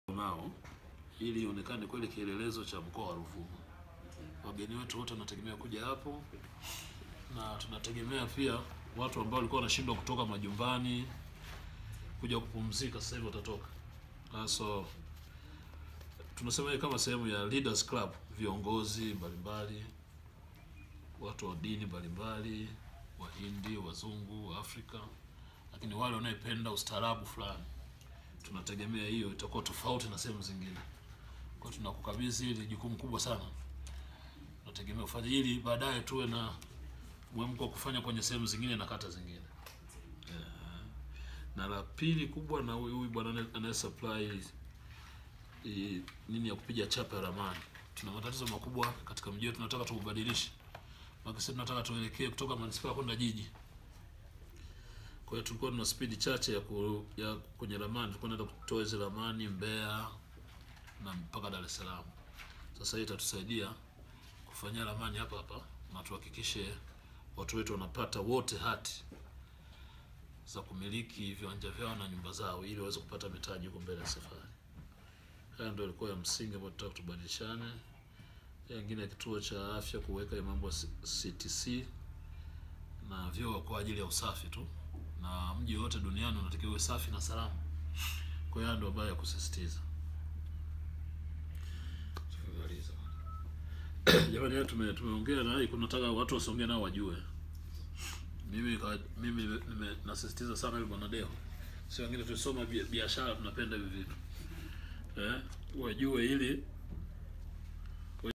MANISPAA YA SONGEA YASAINI MKATABA WA UENDESHAJI BUSTANI
HALMASHAURI ya Manispaa ya Songea mkoani Ruvuma Septemba 13,2018 imesaini mikataba minne ukiwemo mkataba wa uendeshaji wa Bustani ya Manspaa ya Songea ambao umesainiwa bainia ya Halmashauri ya Manispaa ya Mwendeshaji aliyeshinda zabuni anayeitwa Neema Kajange.
Hafla ya kusaini mikataba hiyo imefanyika katika ofisi ya Mstahiki Meya wa Halmashauri ya Manispaa ya Songea Alhaj Abdul Hassani Mshaweji mbele ya wanahabari.
Mkataba wa uendeshaji wa bustani ya Manispaa ya Songea ni wa shilingi milioni 15 ambao utafanyika katika kipindi cha miezi kumi ambapo Mwekezaji ataendesha shughuli mbalimbali  ikiwemo hoteli,choo na mchezo wa bembea.
Kwa mujibu wa Mwekezaji wa Bustani hiyo,bustani hiyo inafunguliwa rasmi Jumatatu ya Septemba 17,2018 na kwamba uzinduzi rasmi wa bustani unatarajia kufanyika Oktoba 27,mwaka huu katika viwanja vya bustani hiyo.
Mkataba wa pili ambao umesainiwa baina ya Halmashauri ya Manispaa ya Songea na Kampuni ya STUMAK ENGINEERING CO.LTD ya Songea unahusu ujenzi wa kituo cha kutolea huduma za afya(CTC)  katika kituo cha afya Mjimwema.Mkataba huo ni wa siku 30 wenye thamani ya shilingi milioni 29,673,311.32.
Mkataba wa tatu ni wa ununuzi wa mashine ya kuchapisha  ramani (PLOTTER MACHINE) ambao umesainiwa baina ya Halmashauri ya Manispaa ya Songea na Kampuni ya DON TECHNOLOGIES LTD ya Songea kwa siku 45 wenye thamani ya shilingi milioni 28,900,000.00
Mkataba wa nne ambao umesaini ni kati ya Halmashauri ya Manispaa ya Songea na Robert Mgowole unaohusu uendeshaji wa choo cha soko la Manzese B wenye thamani ya shilingi milioni 3,600,00.00 katika kipindi cha miezi 10.
Akizungumza mara baada ya kusainiwa kwa mikataba hiyo,Mstahiki Meya wa Manispaa ya Songea Alhaj Abdul Hassan Mshaweji amewaasa wote waliosaini mikataba hiyo kufanyakazi kwa ufanisi kwa sababu Manispaa ya Songea hivi sasa ipo katika mchakato wa kutoka katika Halmashauri ya Manispaa ya kuingia katika Halmashauri ya Jiji.
Imetolewa na Albano Midelo
Afisa Habari Manispaa ya Songea
Septemba 13,2018